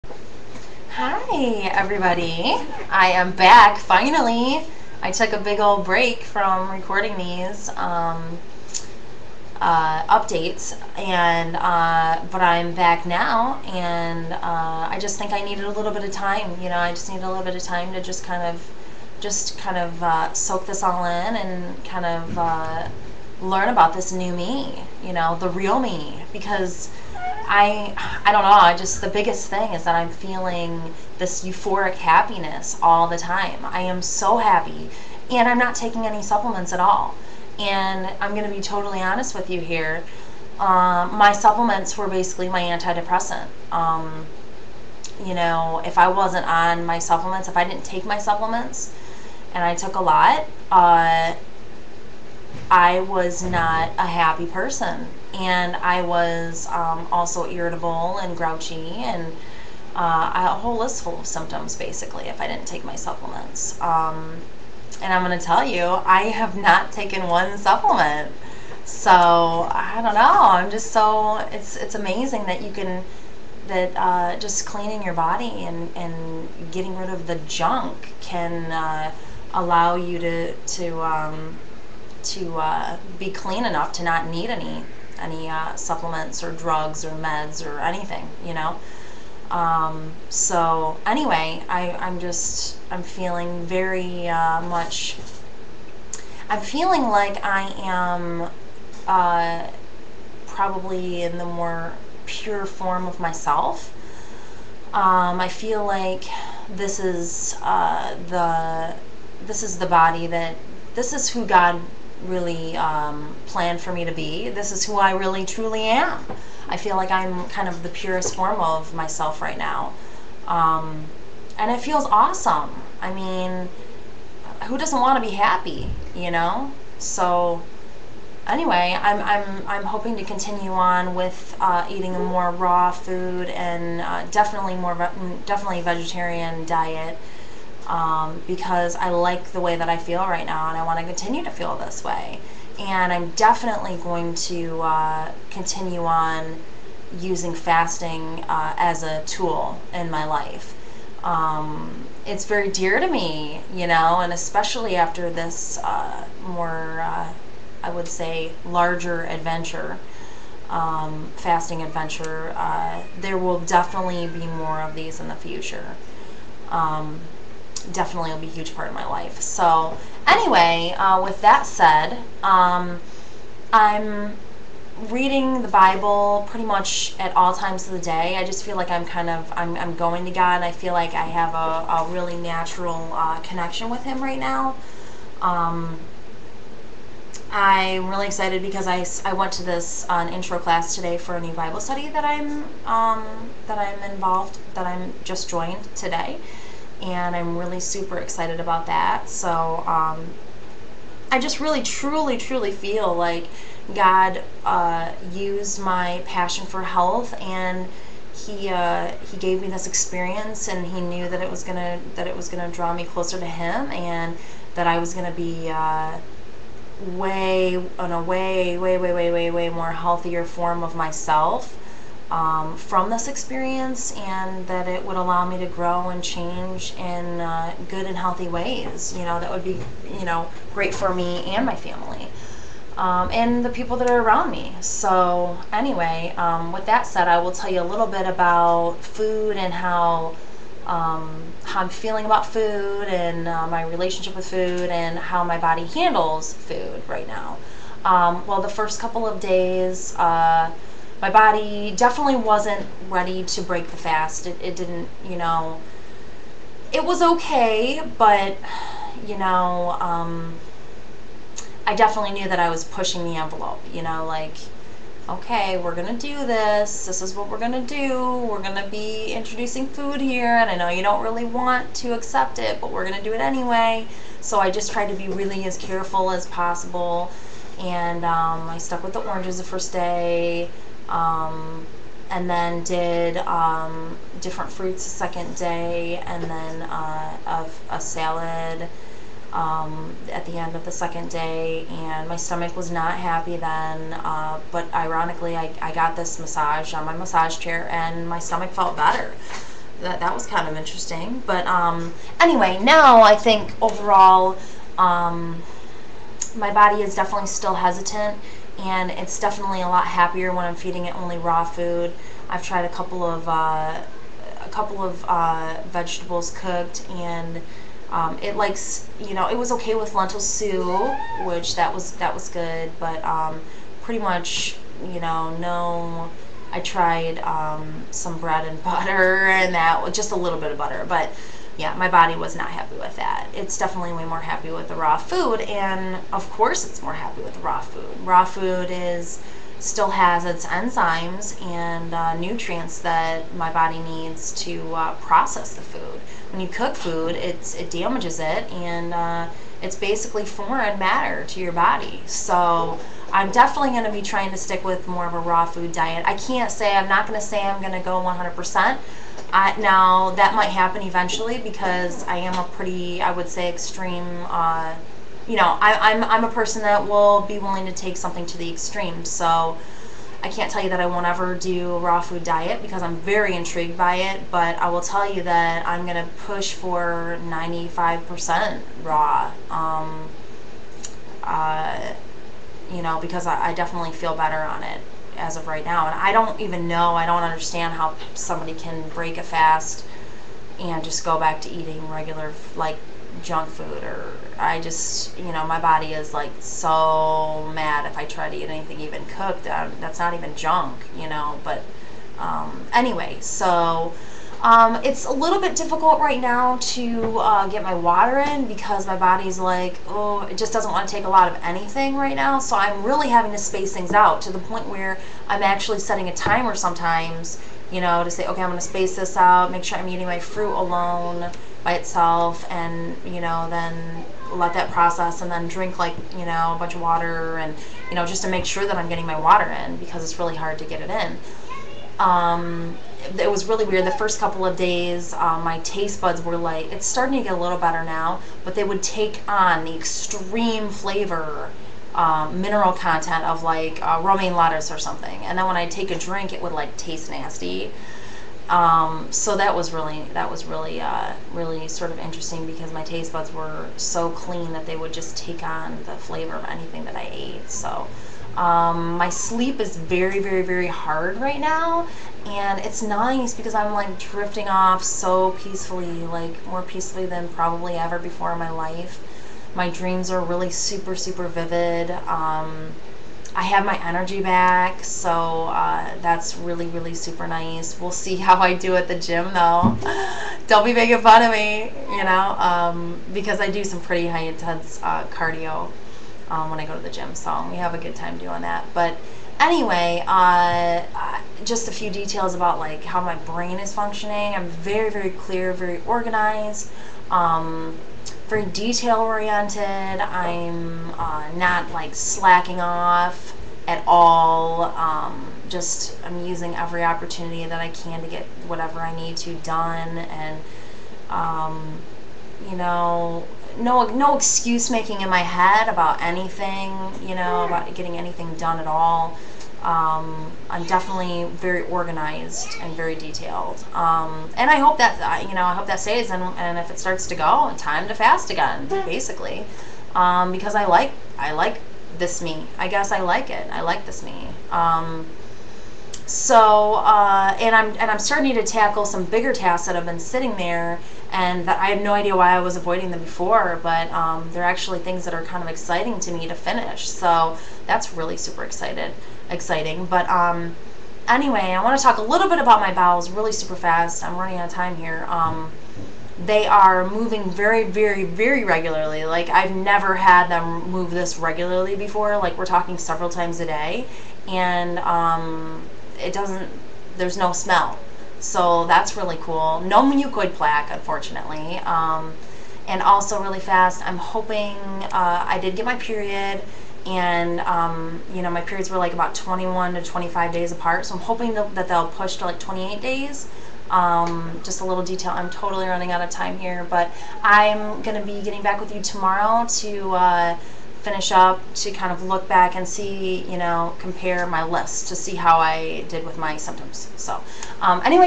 30 Day Water Fast Journey- Post Fast Update- Happy!!!!!!
Here is a little update from my 20 day water fast! What an incredible experience! I am so happy and fruitful right now! Fasting will definitely be a huge part of my life forever!